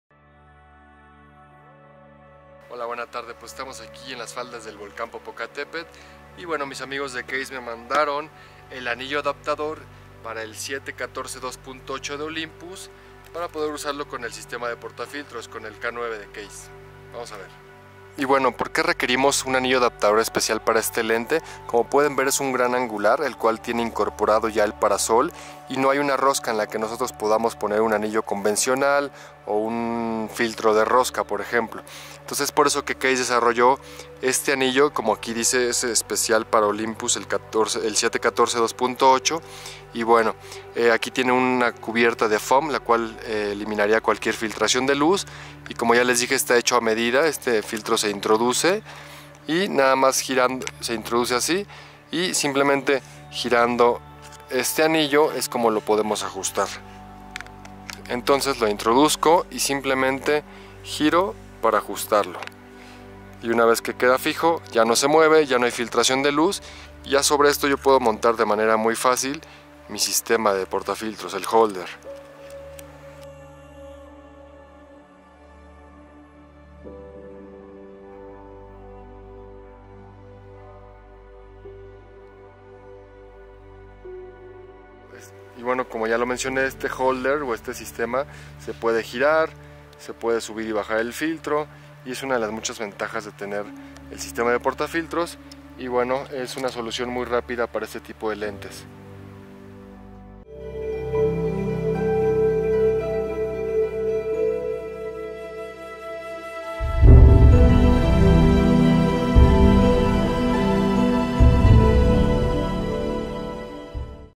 Kase K9 Adapter Ring for Olympus 7-14mm F2.8 lens
The Olympus 7-14mm F2.8 lens can be used on our K9 holder!
Our ambassador Enrique Barquet is showing simple steps of how to use this Adapter Ring for Olympus 7-14 F2.8 lens in the field!